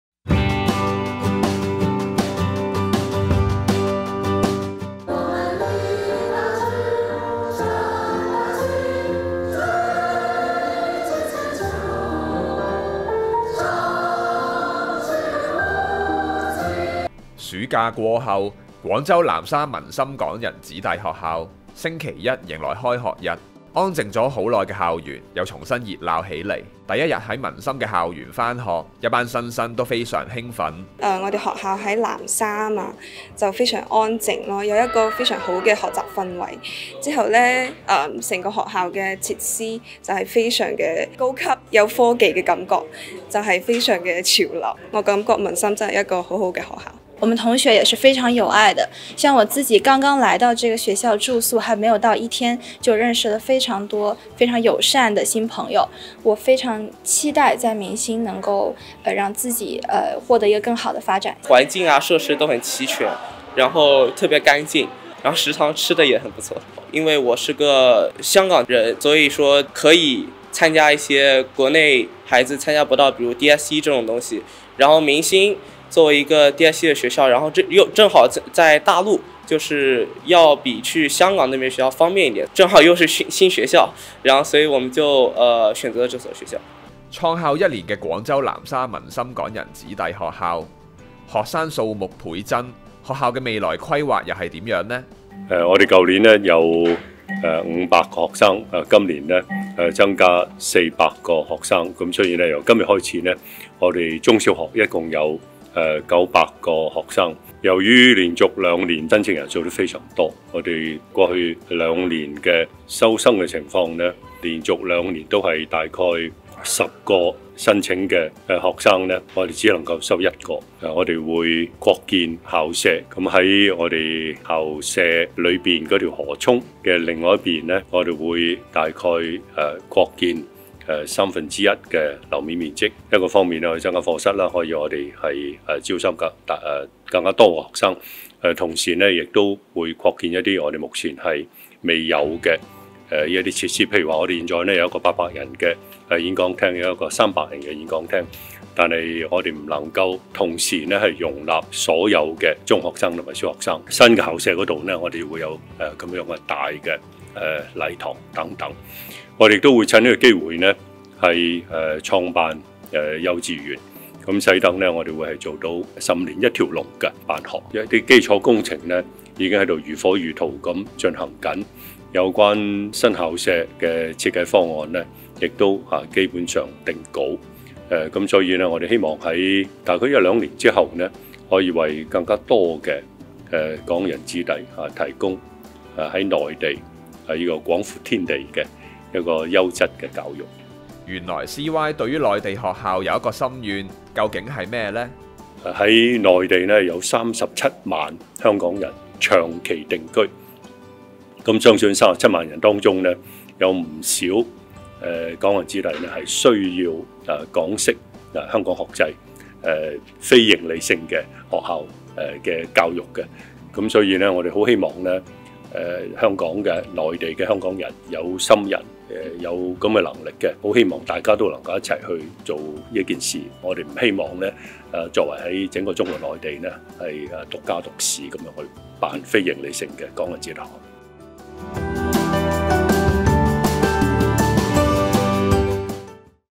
【短片】【民心開學】廣州南沙民心港人子弟學校開學 新生雀躍大讚校園、梁振英談發展宏圖：擴建校園、辦幼稚園
//由CY牽頭成立嘅廣州南沙民心港人子弟學校尋日開學喇！一班新生對新學期充滿期待，CY仲介紹咗民心嘅發展大計，一齊睇片！//